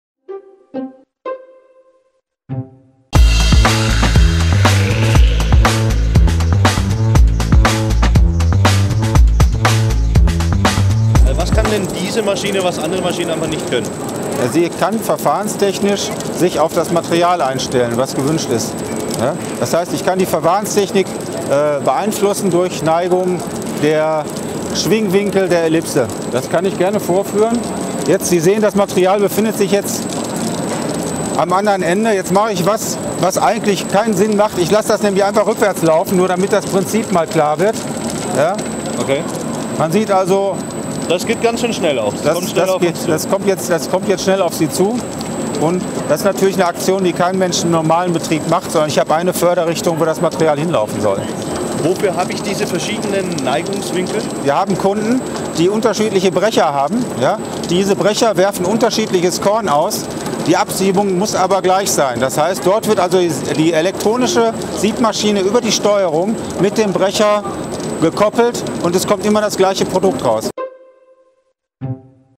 Siebtechnik • Siebmaschinen mit individuell einstellbarer Verfahrenstechnik
Eine Maschine die sich verfahrenstechnisch auf ihr Materialgut einstellen kann? Wie praktisch. Je nach Abbau gibt es unterschiedliches Korn dass ebenso abgetrennt werden muss.

Wie sich das technisch von der Firma Siebtechnik umsetzen lässt, haben wir auf der bauma in München recherchiert!

Interview mit Jürg Pollmanns auf der bauma 2016.